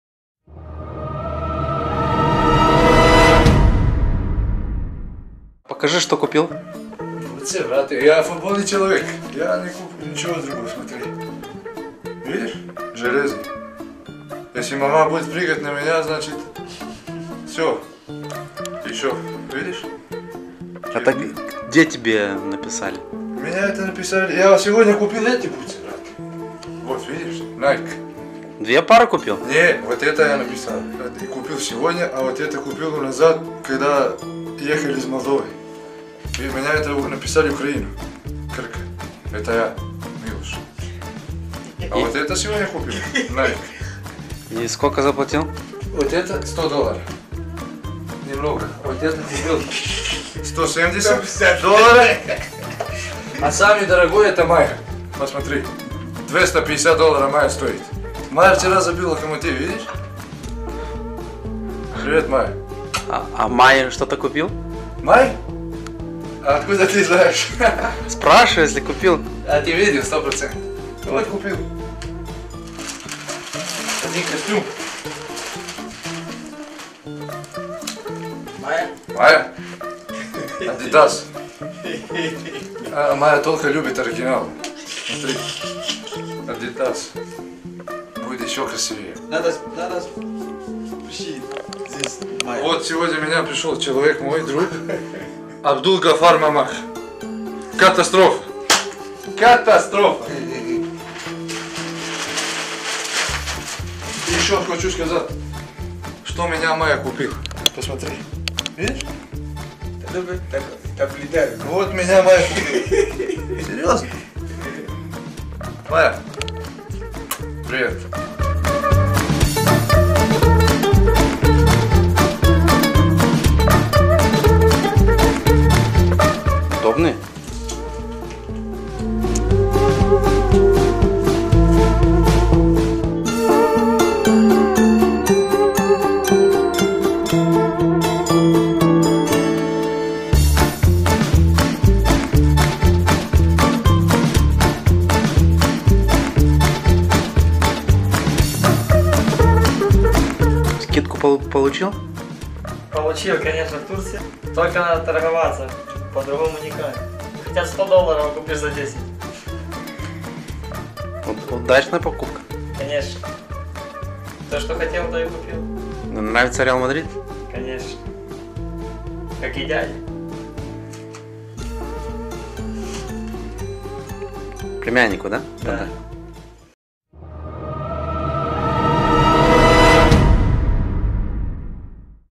(FUNNY moments) Camp for Champions, Day 19 (24.02.15) Dacia Chisinau, Turkey, Antalya, Lara
Fotbaliştii au celebrat victoria cu «Loko» la shopping (Video)